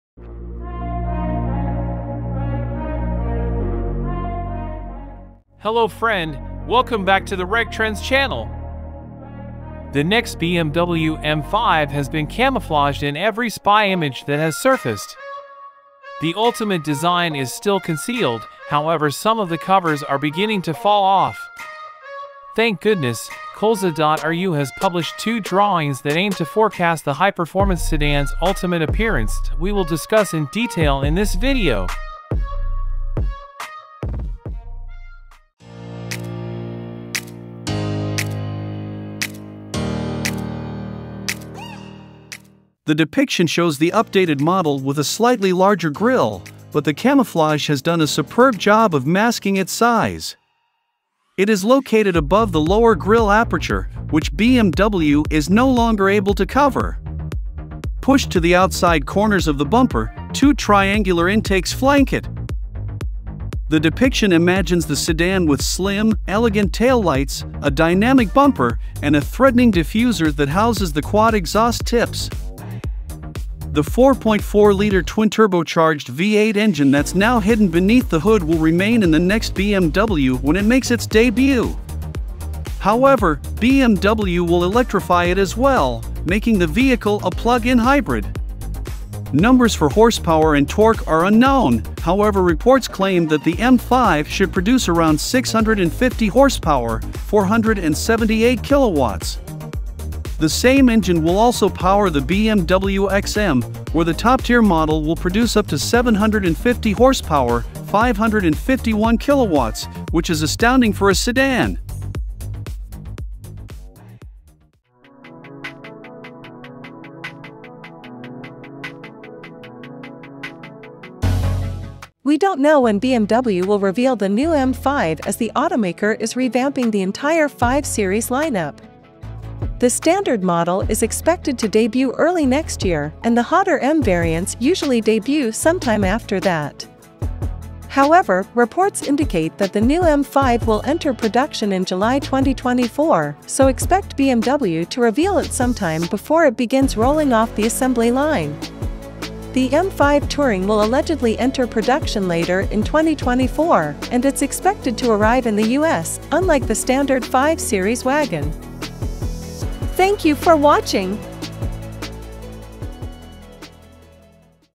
New 2024 bmw m5 competition - bmw m5 2024 specs, hybrid, sound | release date, interior & exterior
2024 bmw m5 competition - The depiction shows the updated model with a slightly larger grille, but the camouflage has done a superb job of masking its size. It is located above the lower grille aperture, which BMW is no longer able to cover. Pushed to the outside corners of the bumper, two triangular intakes flank it.

The depiction imagines the sedan with slim, elegant taillights, a dynamic bumper, and a threatening diffuser that houses the quad exhaust tips.